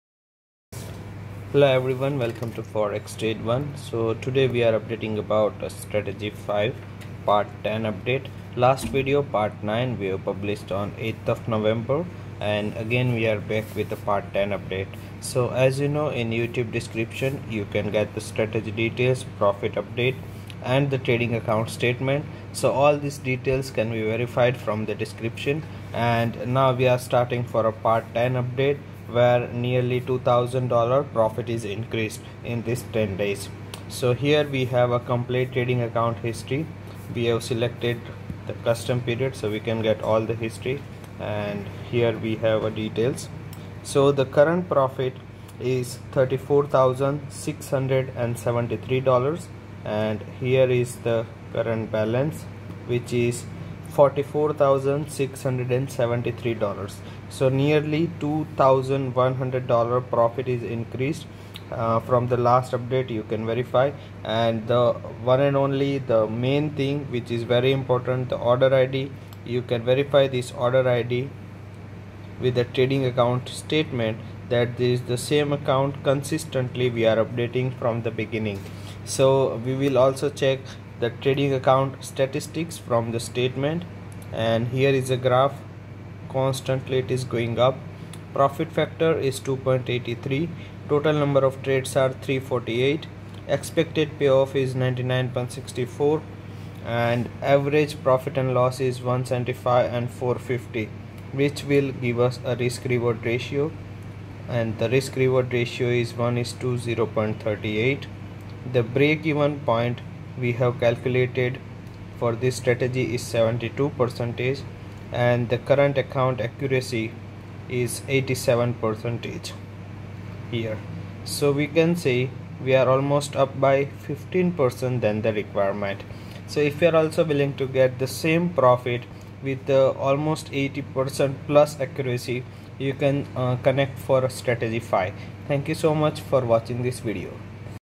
STRATEGY 5 Calendar Data Trading Strategy - 21% ROI Increased in 10Days with Forex Trading PART 10
Chapter
00:00 Introduction
00:10 Verify Previous Update
00:40 Trading Account History
01:40 Trading Account Statement

Mostly traded in following pairs:
XAUUSD
GBPUSD
USDCAD
EURUSD
USDJPY
NZDUSD
USDCHF
AUDUSD